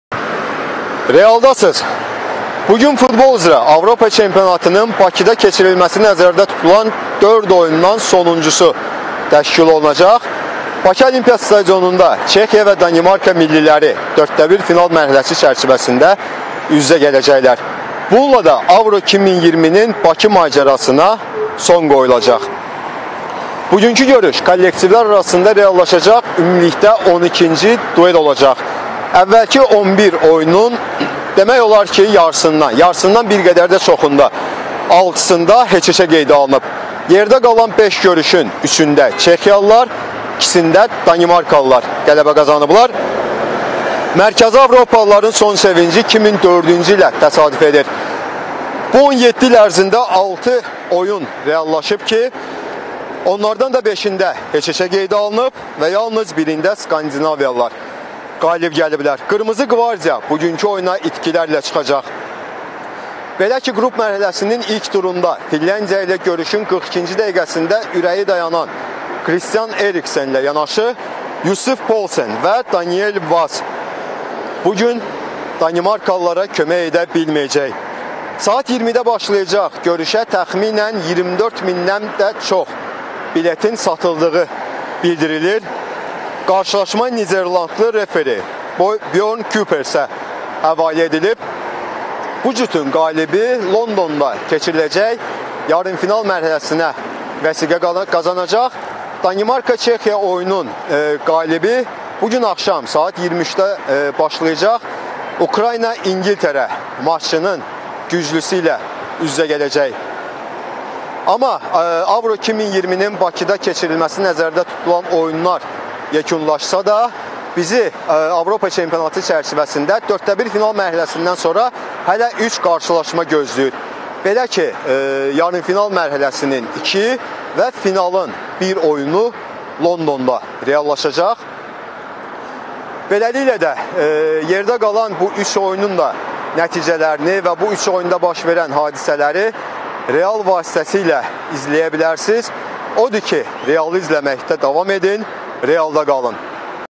Paytaxtda son duel
Çexiya və Danimarka Londona bileti Bakıdan alacaq